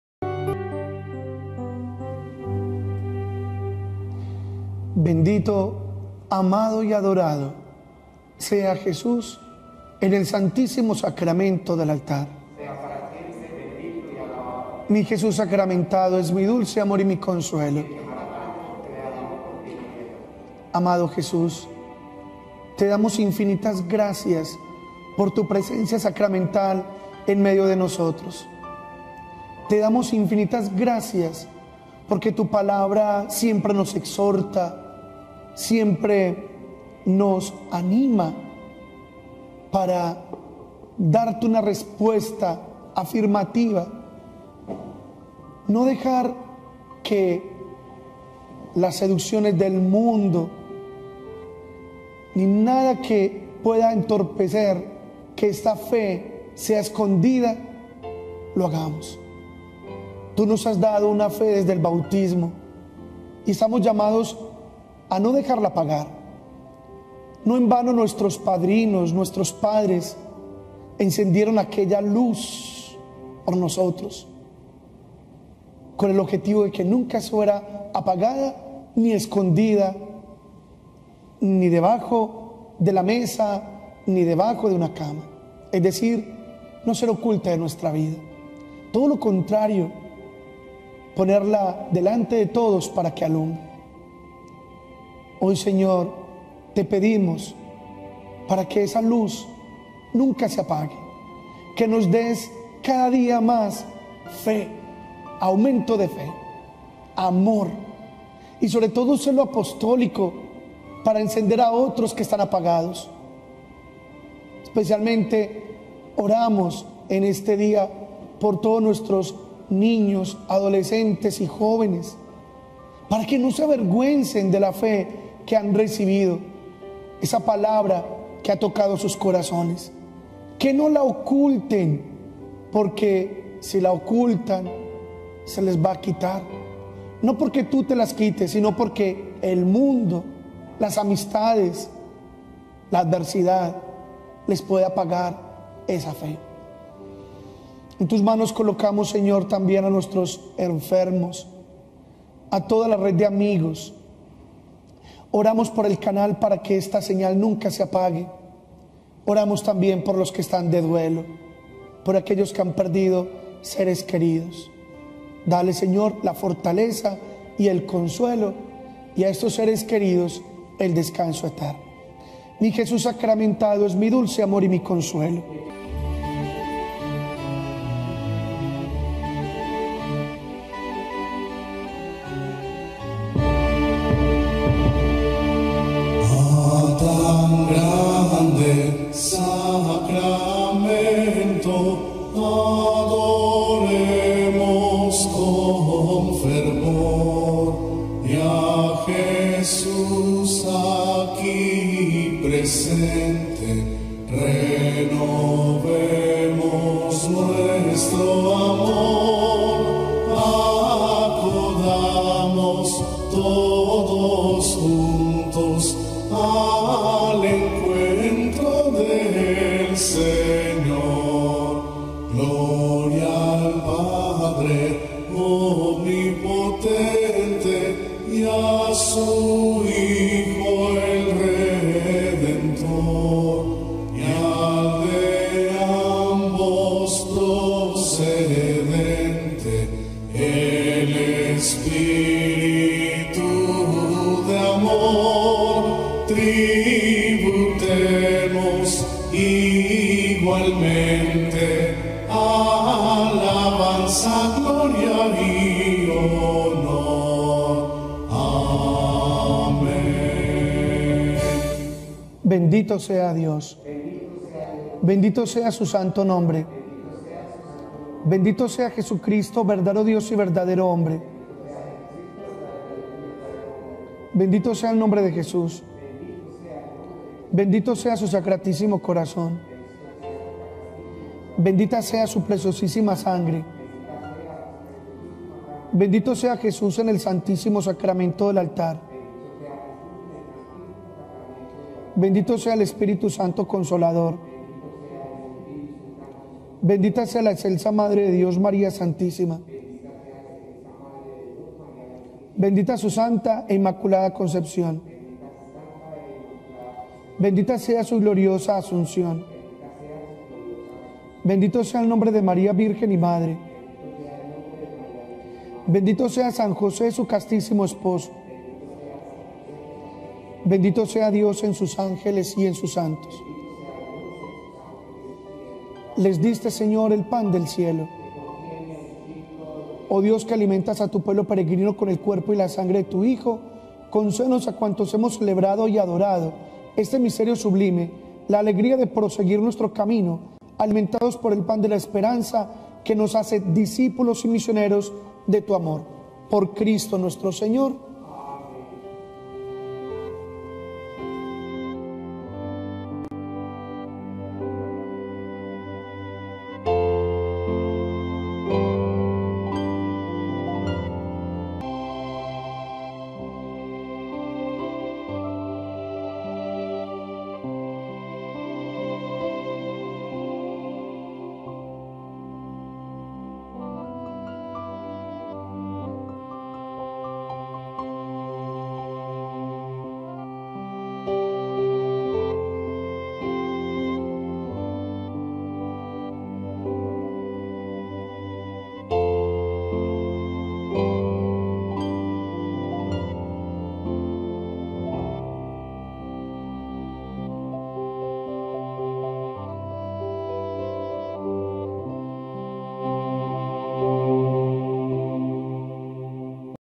Adoración Eucarística ,Él tiene la disposición de sanar tus heridas.
“La eucaristía es, por excelencia, el sacramento de la fe, que exige de ti la actitud del niño indefenso, la actitud del pecador que nada desea con más ansiedad que la sanación de su mal.”
 Si deseas puedes compartir el Evangelio y darle me gusta.